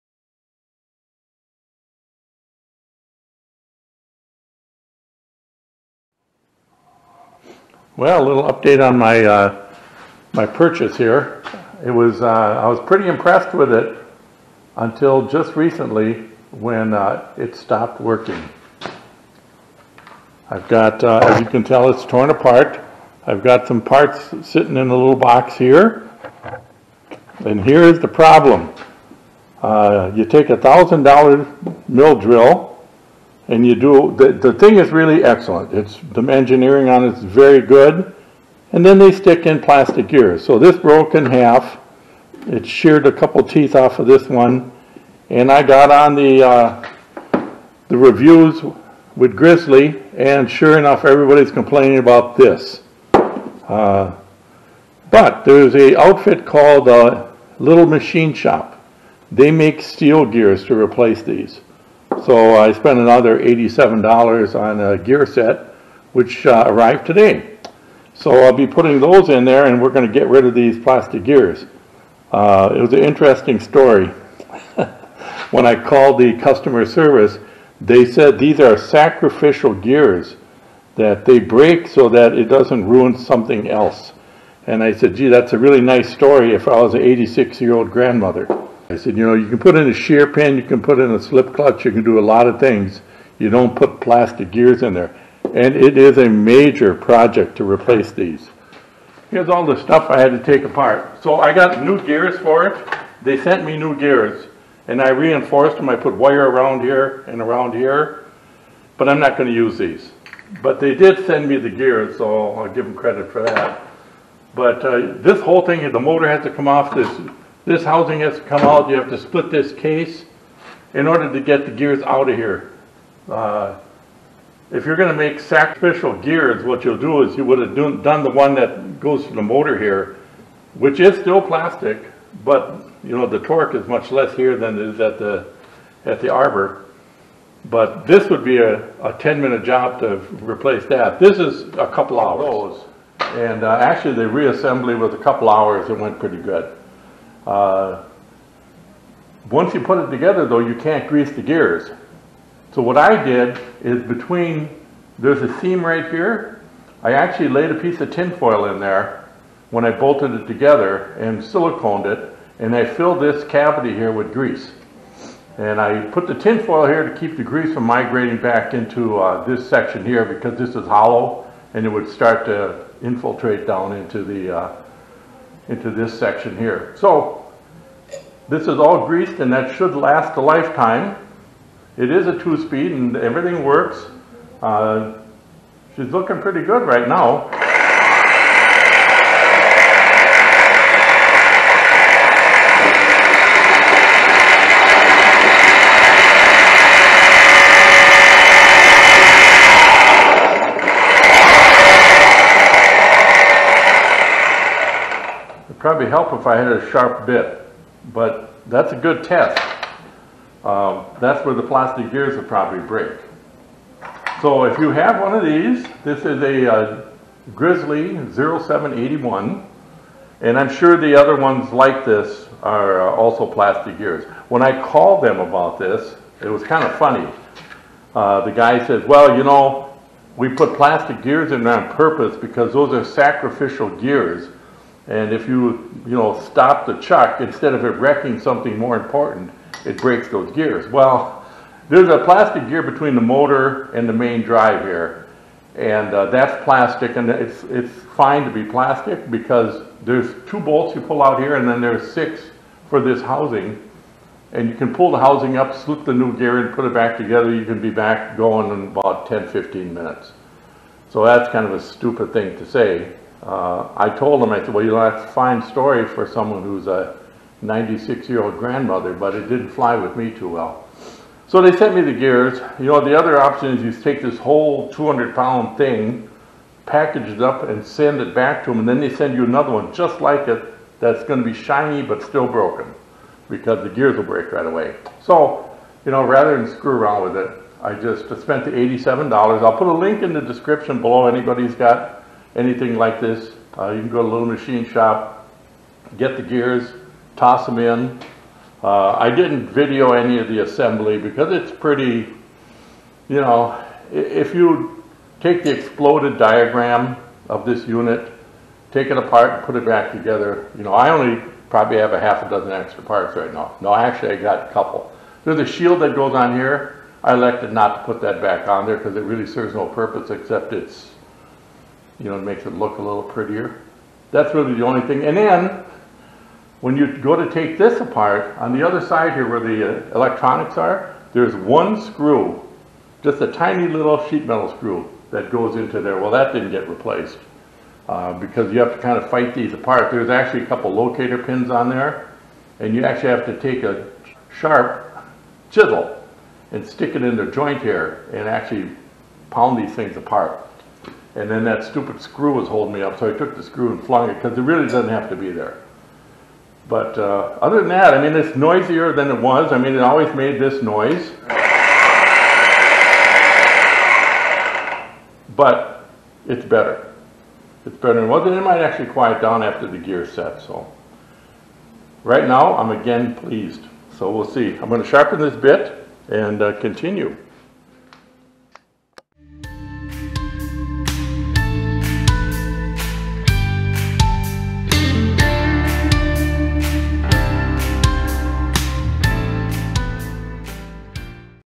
Grizzly G0781 Mill Drill plastic gear problems
Common problem with Grizzly mini Drill/Mill - model G0781 and G8689 and G0758. Plastic gears breaking.